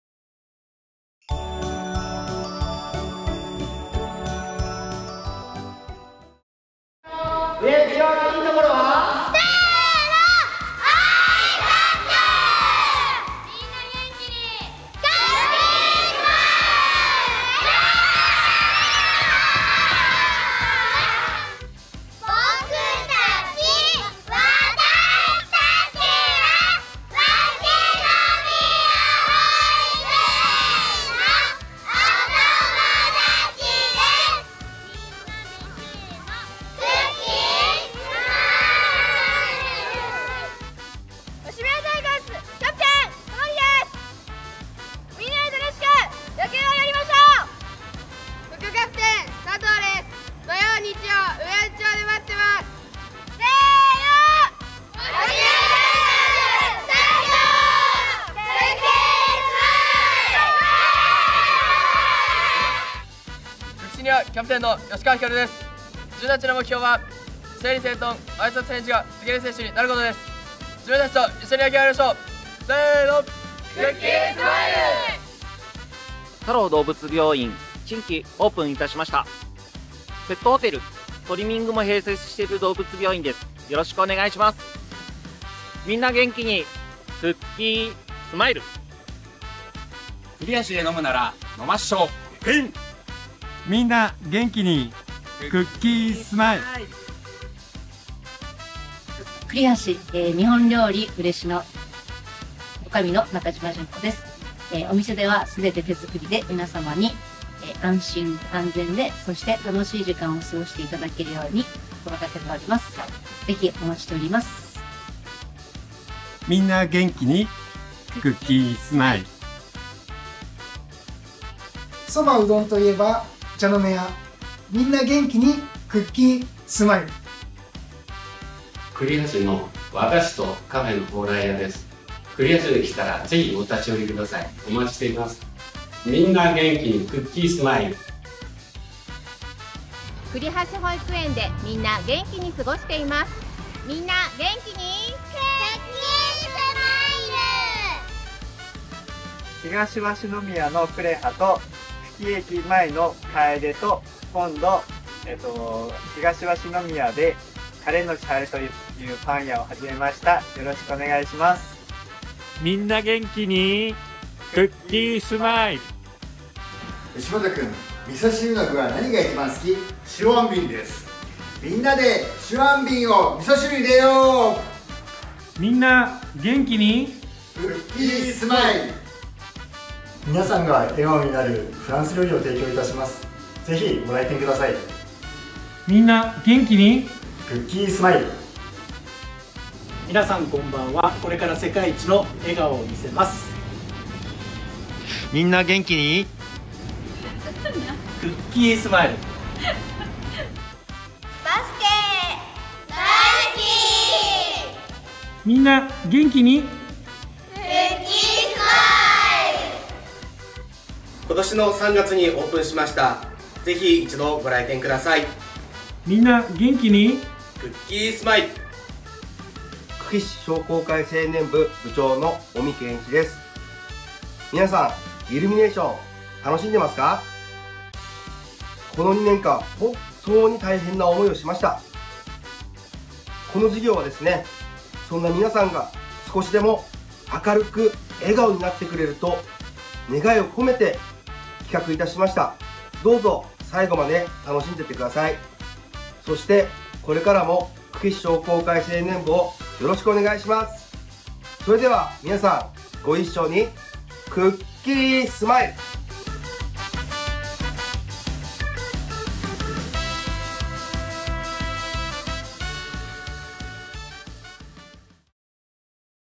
クリスマスイルミネーション メッセージ動画 久喜市商工会青年部
久喜駅東口 ロータリーで開催されているクリスマスイルミネーションで上映されているメッセージ動画です。

★クリスマスイルミネーション
 点灯期間12/12~12/25（17：00~24：00）

★メッセージ動画上映
 12月19日と12月25日のみ

久喜市商工会青年部